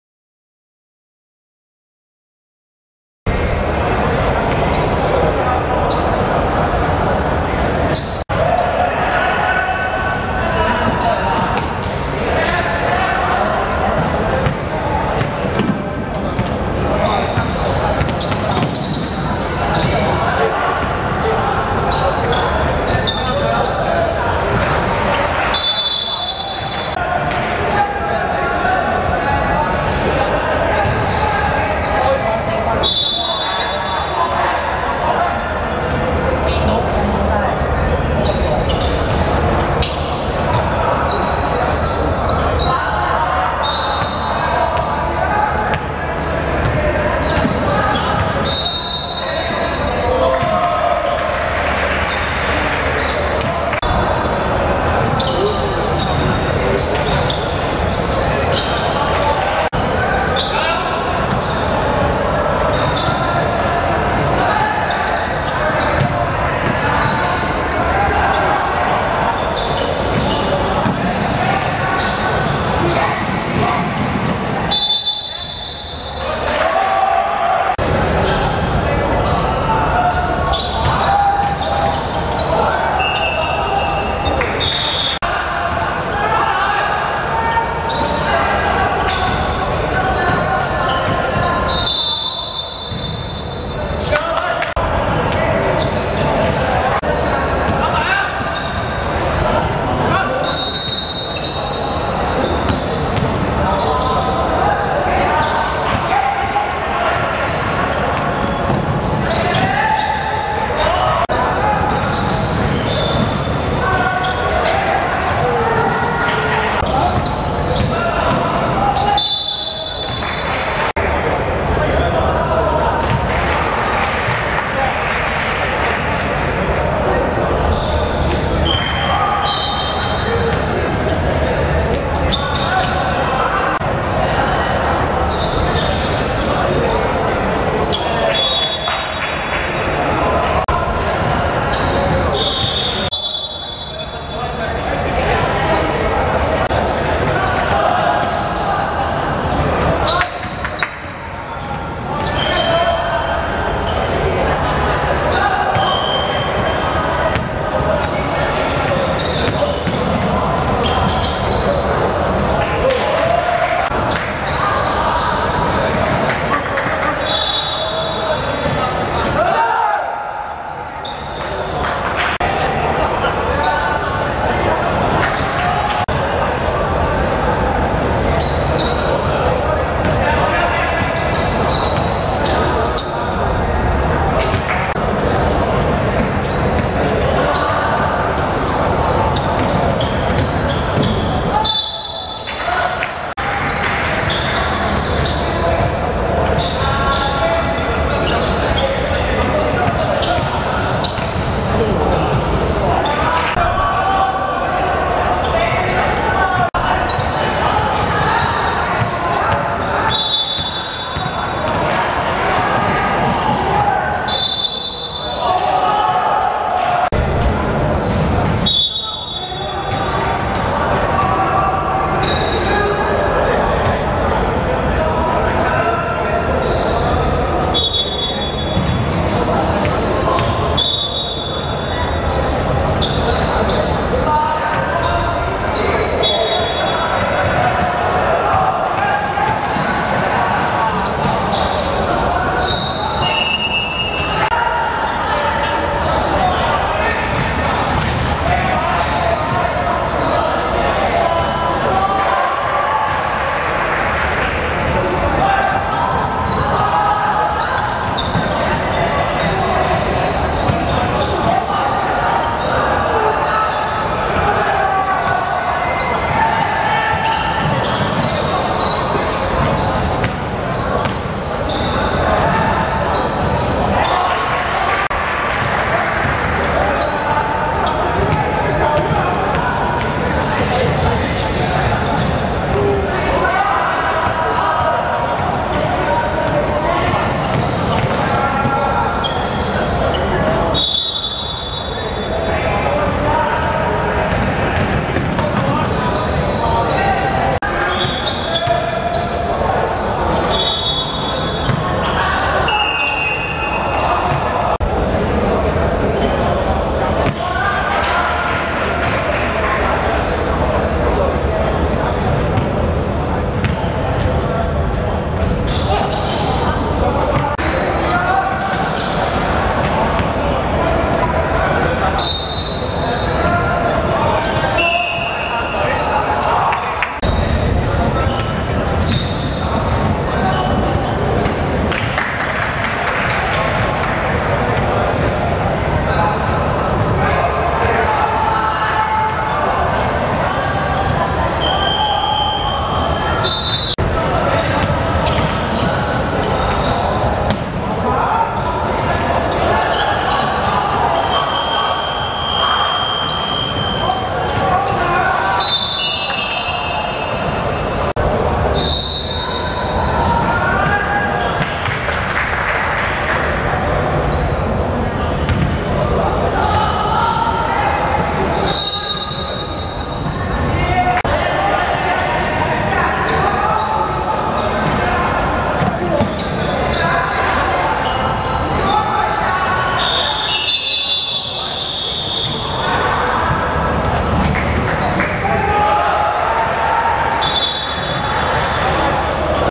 楊葛小琳vs迦密主恩(2016.6.30.學界籃球馬拉松~男子分組賽)精華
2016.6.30.港灣道體育館 15:45 楊葛小琳(藍) 27 : 22 迦密主恩(紅)